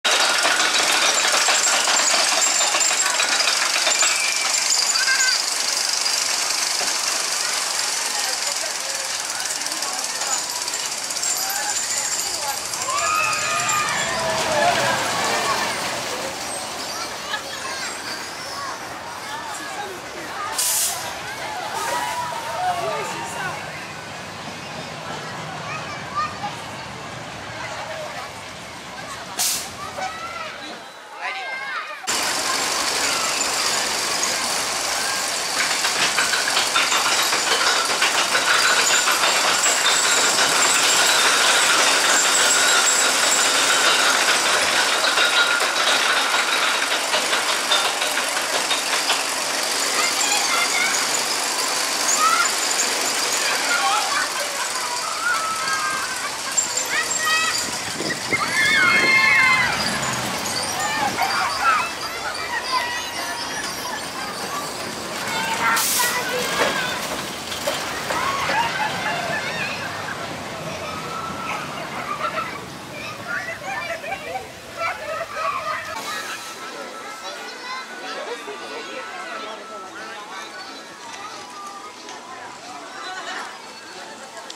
Fils du Dragon Off-Ride Footage, Jardin d'Acclimatation Kiddie Coaster | Non-Copyright
In early 2023, Jardin d'Acclimatation closed the Dragon Chinois kiddie coaster.  While they build a bigger Dragon coaster for the 2024 season, the park added a temporary kiddie coaster named Fils du Dragon.  This is a Kılıç Lunapark Dragon coaster.